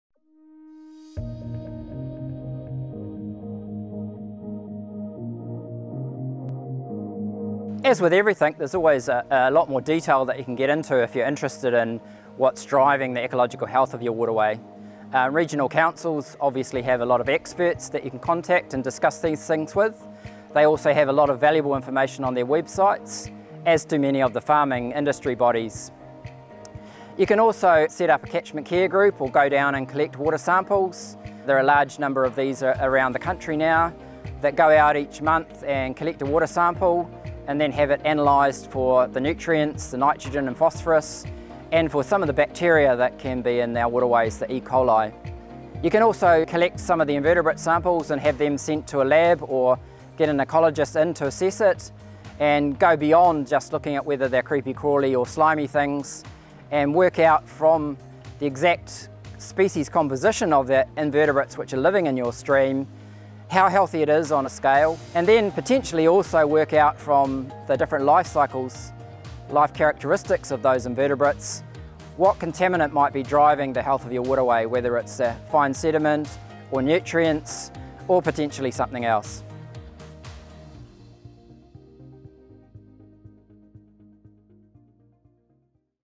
Stream Health series Video 4: Further Monitoring Options
There are lots of ways to assess freshwater ecosystem health and Dr Death takes us through a number of different methods farmers can use to assess their waterway health in greater detail.

Whilst it is suggested the Stream Health Check is done in summer during low flows when the waterway is most stressed, it is also important to observe your waterways after heavy rain or adverse weather events as this may identify other unidentified critical source areas.  It is also important to consider where you are in the catchment, a headwater stream can have good Ecological Health but could still carry contaminants that are higher than background levels and potentially have a cumulative effect on downstream receiving bodies.